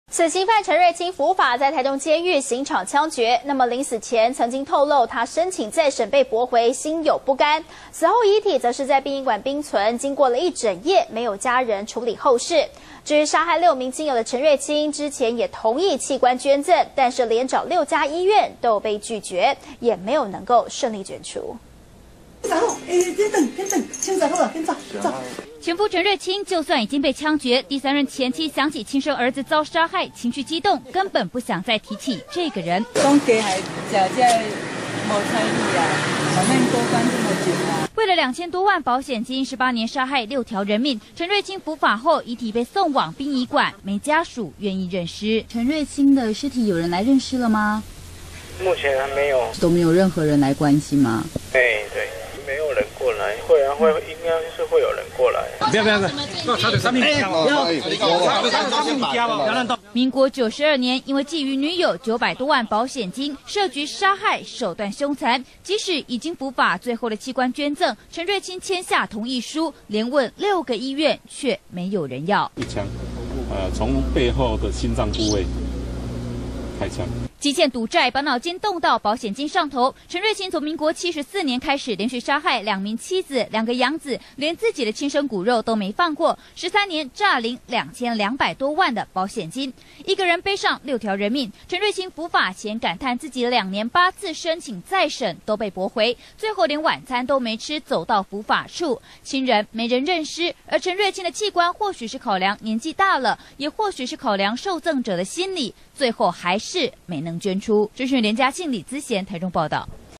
中天新聞》殺兩妻三兒 陳瑞欽遺體沒人領
中天新聞》死刑犯陳瑞欽伏法，在台中監獄刑場槍決，臨死前，透露曾聲請再審被駁回，說他心有不甘。死後遺體在殯儀館冰存，經過一整夜，沒有家人處理後事，陳瑞欽也同意器官捐贈，但連找6家醫院，都被拒絕，沒能順利捐出。